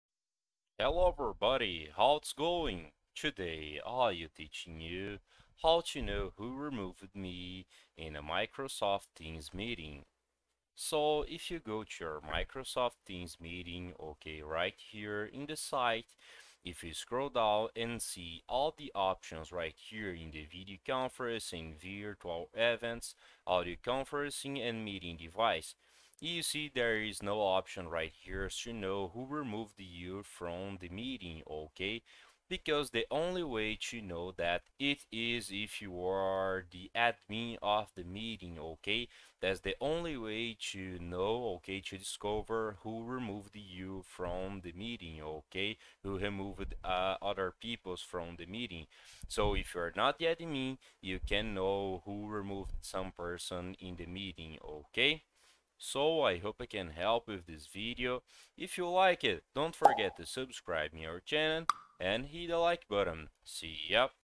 How To Know Who Removed Me From Microsoft Teams Meeting (EASY!)
In this video I will show you how to know who removed me from microsoft teams meeting.

I hope I have helped you with the video!